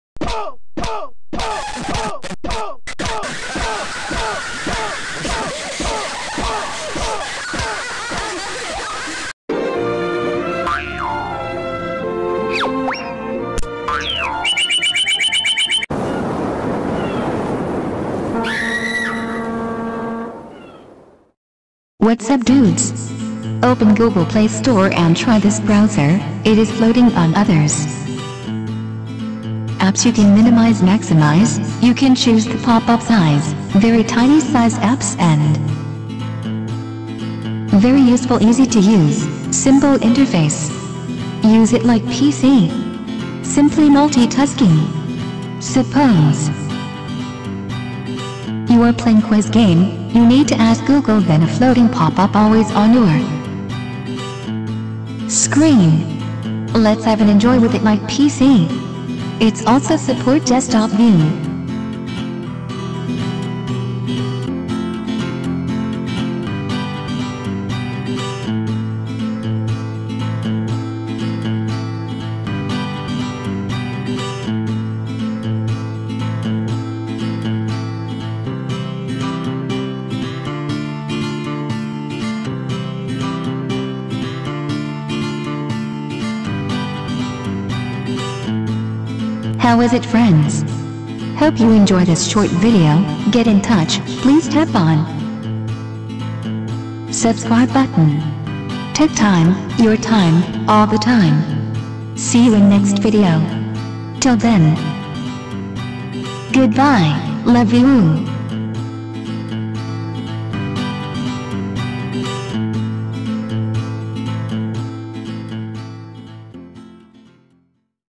Browse web in small pop up window,like PC. when using other apps or games.
This small mb tiny browser can draw over other apps.you can browse any time when using other apps.it can be minimize maximize, its floating on your screen.its also support desktop view.